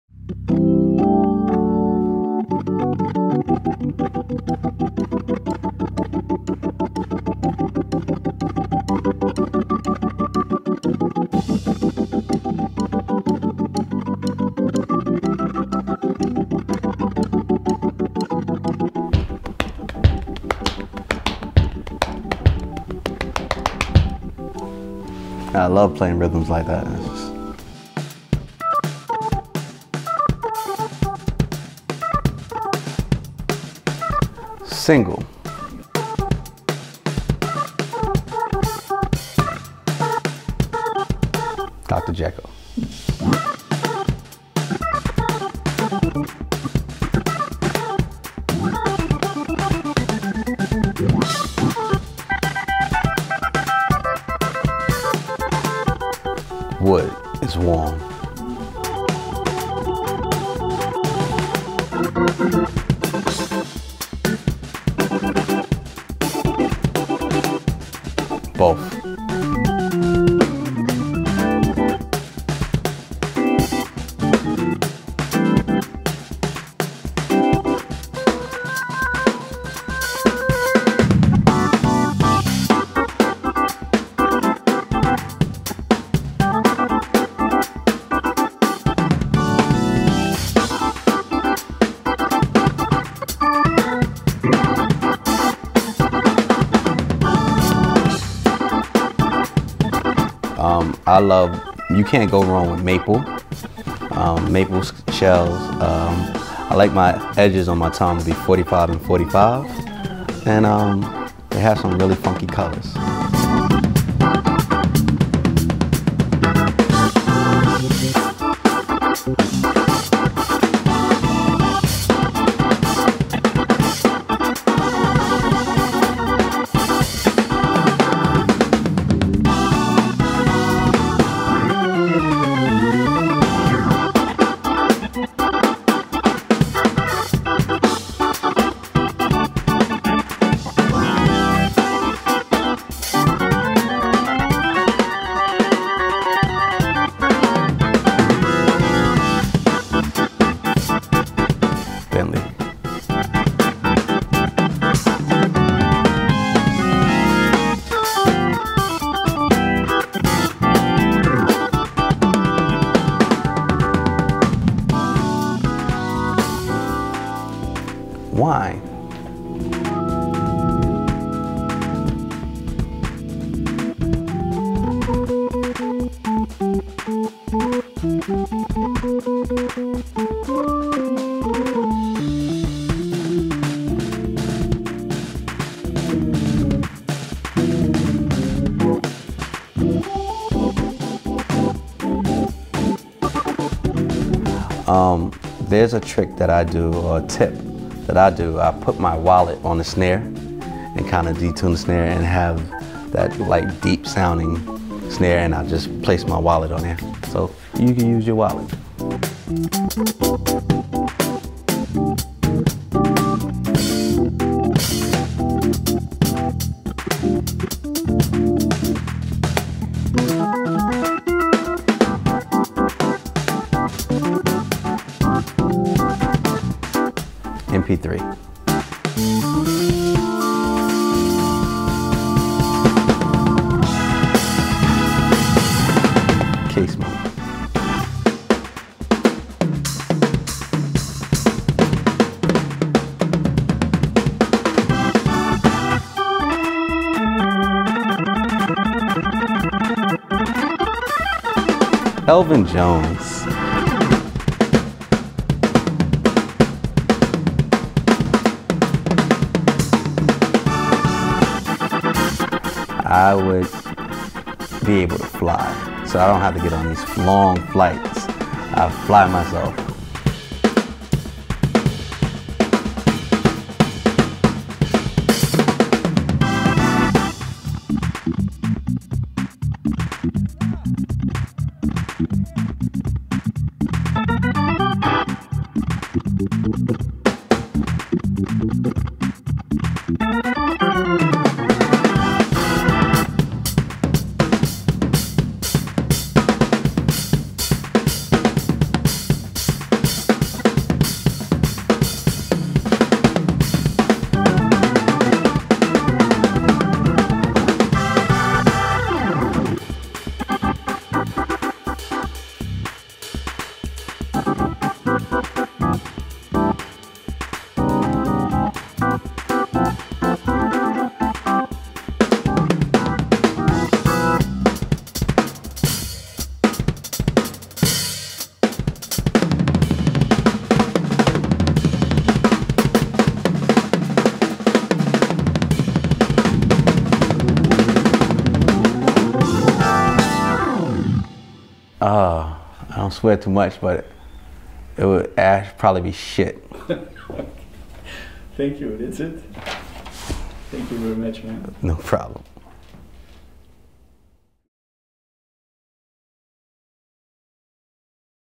Derrick Wright and Quennel Gaskin - DW drums, Paiste @Paris 2018
Derrick Wright and Quennel Gaskin - Music session in Paris 2018.
DW drums and Paiste artist